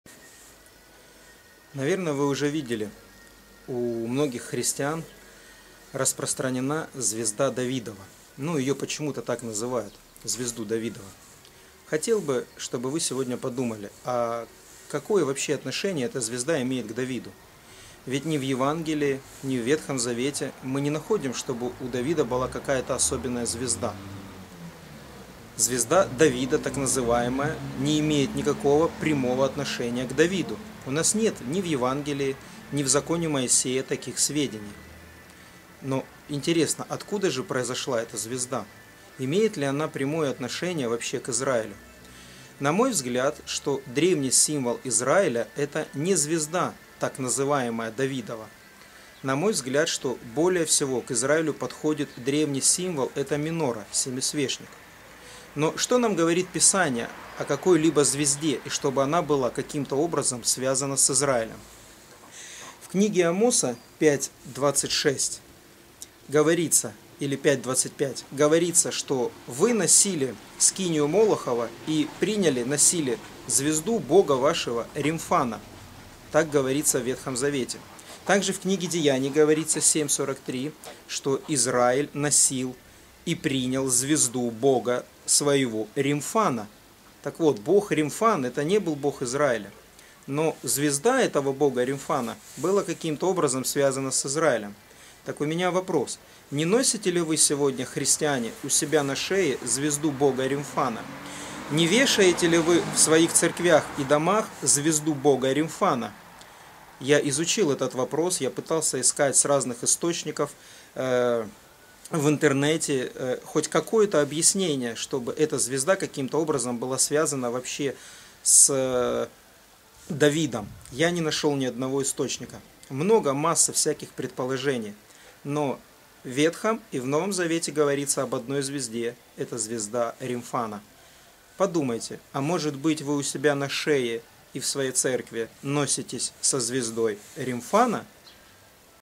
Чья это звезда, Давида или Ремфана?
Сегодня у тысяч христиан в домах и церквах висит "звезда Давида" как символ Израиля и изображение на его флаге,но вы задумывались каким образом эта звезда имеет отношение к Давиду? Вы носили скинию Молохову и звезду бога вашего Ремфана, изображения, которые вы сделали для себя. Амос 5:26